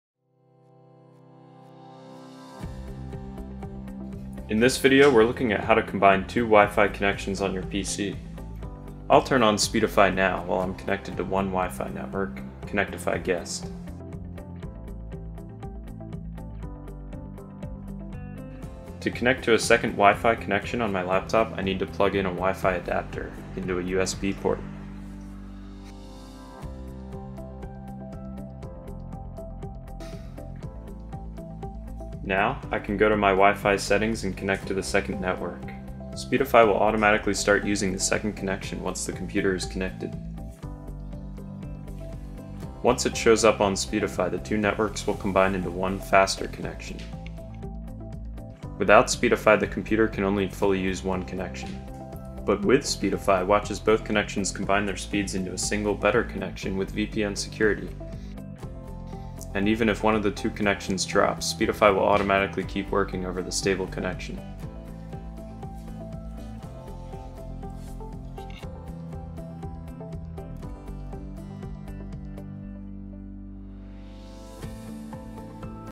How to Combine 2 WiFi Connections on Your Computer
In this video we’re looking at how to combine 2 WiFi Internet connections on your Windows PC or macOS computer so you get increased bandwidth by using both WiFI networks at the same time.

If you have access to 2 WiFi networks from 2 separate upstream networks (sources) - e.g. your own home WiFi connection and a public WiFi nearby - you can combine them in order to get a faster, reliable and more secure Internet connection. Combining 2 WiFi connections from the same network or that go into the same router will not help you.

Here’s how to combine 2 WiFi connections:

Step 2 - Connect to the first WiFi network connection with your laptop’s built-in WiFi adapter or using a WiFi card on your PC / Mac.
Step 3 - Connect to the second WiFi network connection with a USB WiFi adapter.
Speedify will automatically start using both WiFi connections once they’re connected.
Without Speedify, your computer is unable to use both WiFi Internet connections at the same time.

By using Speedify, you’ll get a super-connection to the Internet with higher bandwidth, lower latency, increased reliability and VPN security! Even if one WiFi connection drops, you’ll still be online, as Speedify will work with the remaining working WiFi Internet connection.

What other types of connections would you want to combine? Be sure you chime in below, in the comments section!